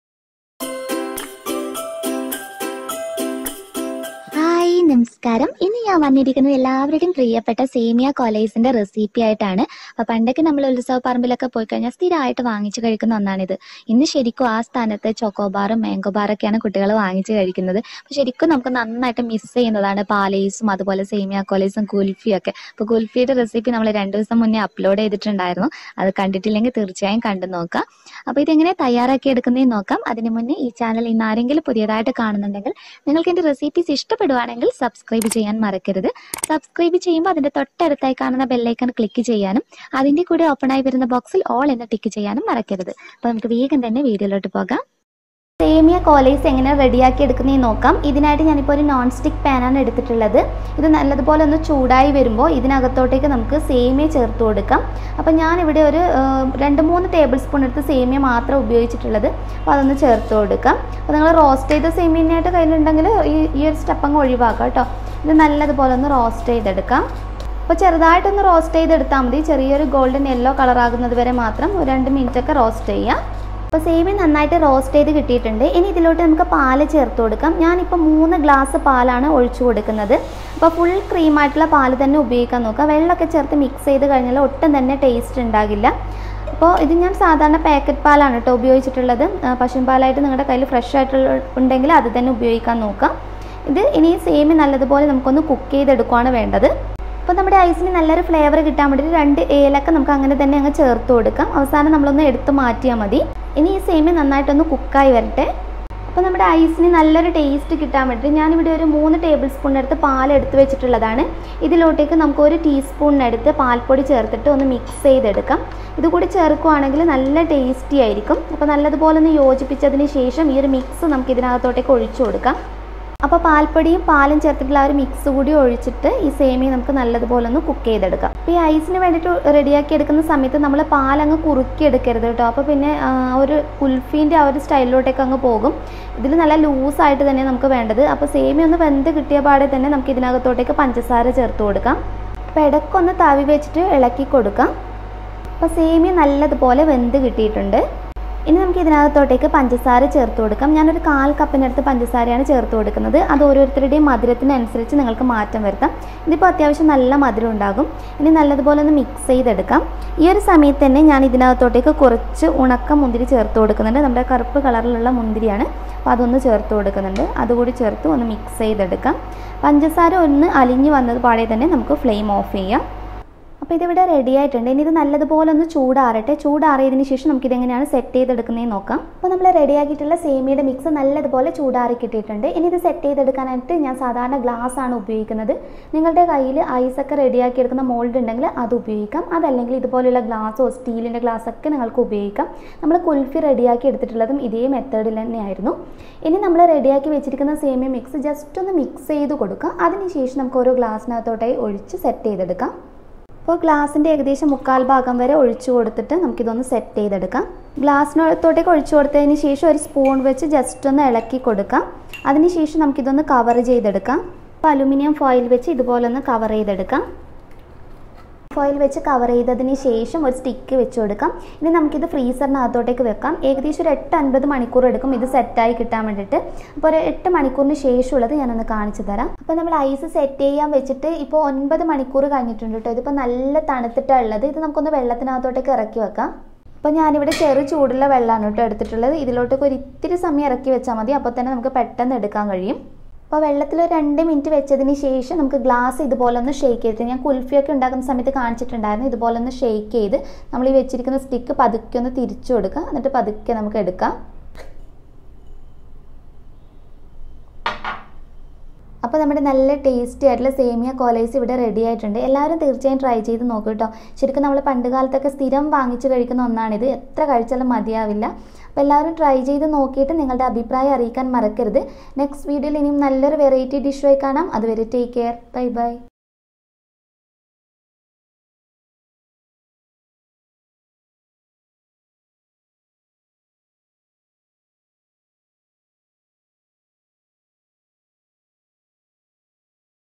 സേമിയ പാൽ ഐസ് എങ്ങനെ എളുപ്പത്തിൽ ഉണ്ടാക്കാം - Semiya Paal Ice സൈക്കിളിൽ വരുന്ന ഐസ് ഫ്രൂട്ട്#145
Authentic Kerala Recipes